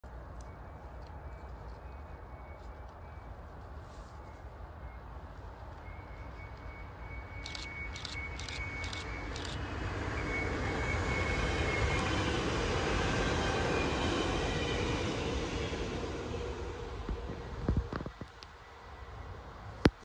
[4K] NS VIRM 9582 with OLD FRONT passes Hengelo!
NS VIRM 9582 passes Hengelo (NL) as InterCity 1743 from Den Haag Centraal (NL) to Enschede (NL). Back then, had this train still the old nose.
Operator/Company: Nederlandse Spoorwegen Reizigers (NSR)
Rolling stock: Verlengd InterRegio Materieel (VIRM)
Vehicle number(s): 9582
Service number (if known): InterCity 1743
Train ride: IC 1743 Den Haag Centraal (NL) → Enschede (NL)
Location: Westermaatsweg, Hengelo (NL)
Recording date video: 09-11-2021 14:02